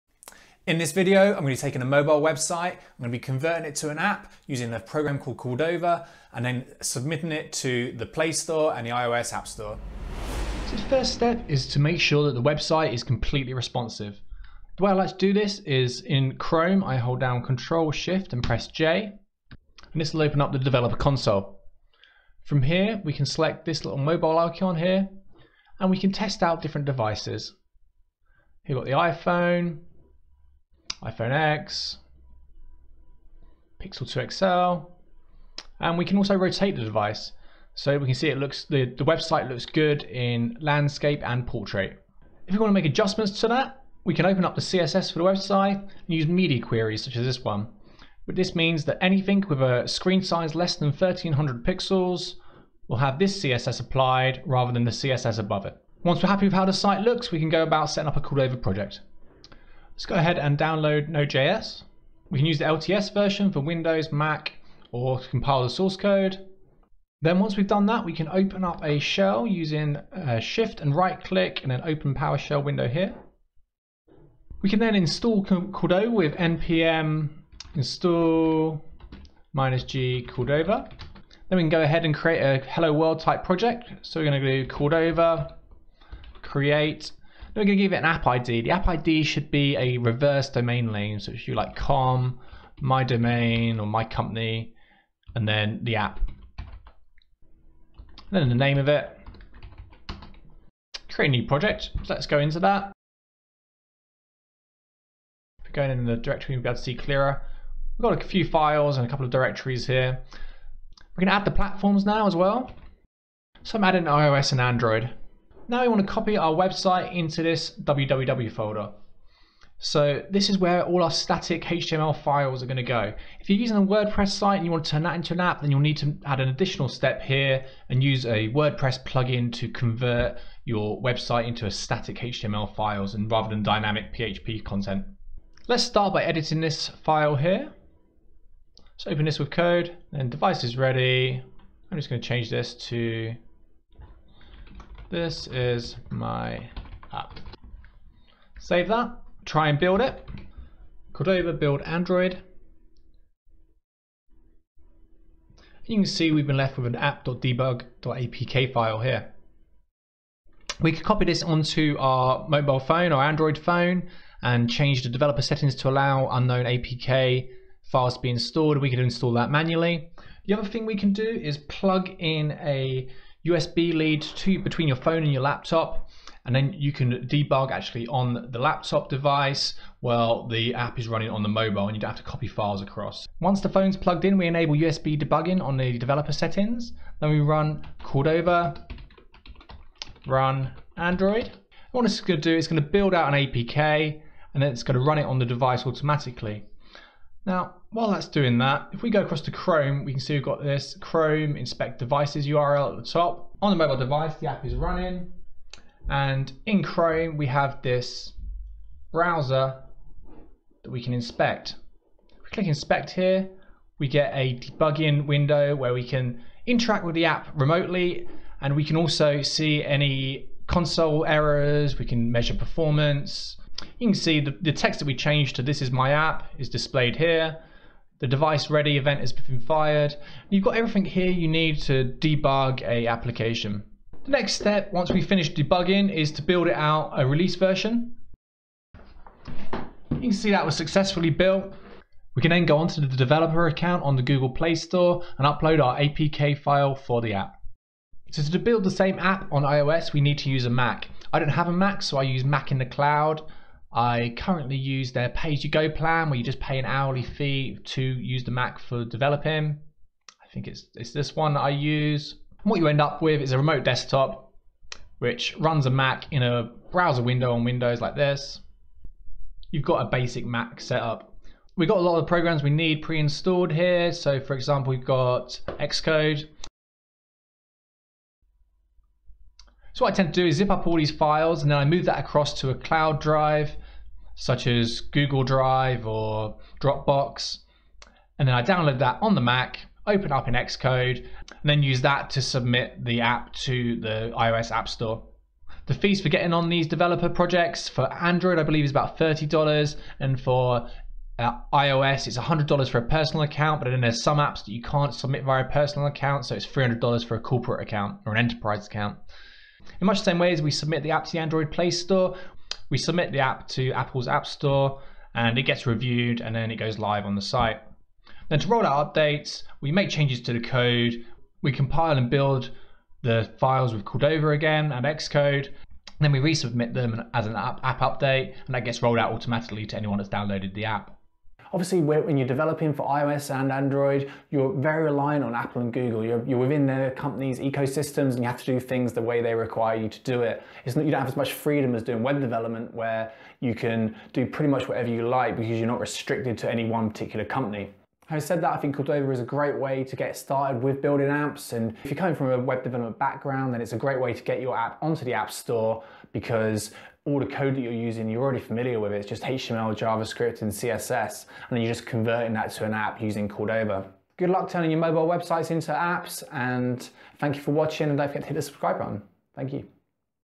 How do you turn a website into an app in 7 steps
This is a step by step guide to using a open-source package called Cordova to turn a website into mobile app on Android and iOS. I’ll be using a windows device for this but all the packages are also available for a Mac. To publish an app to the iOS app store you need to compile on a Mac. I used a cloud service to do this called MacInCloud which lets you rent a Mac on a remote desktop.